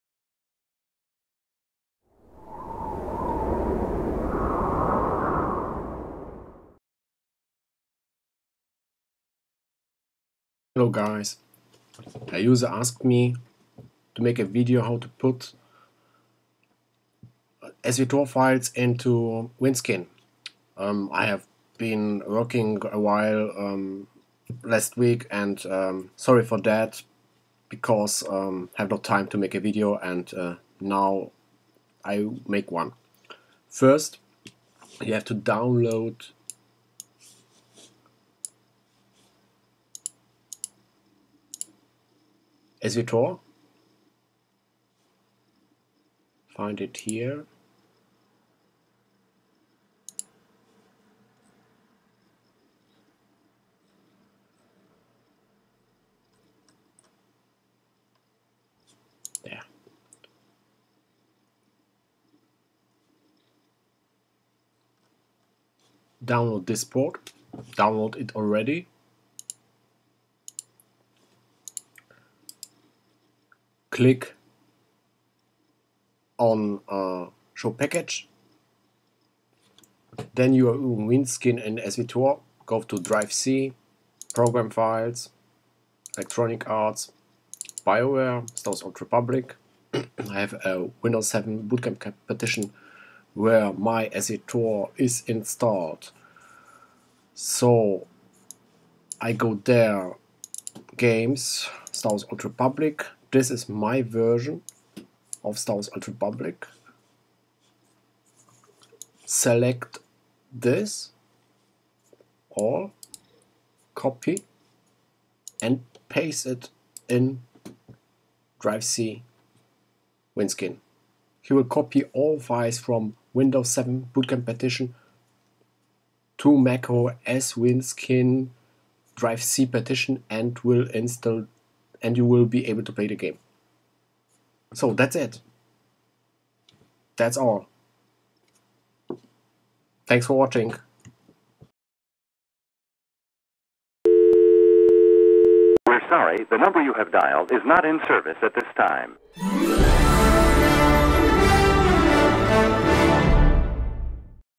SWTOR & WINE HOW TO put your Files in Wineskin
A User asked me to make a video. How to put your SWTOR Files into Wineskin, so I decided to make one. Have a lot of work last week, and I have no time for this, but now finaly it is finished. Enjoy. When you have questions then ask me please.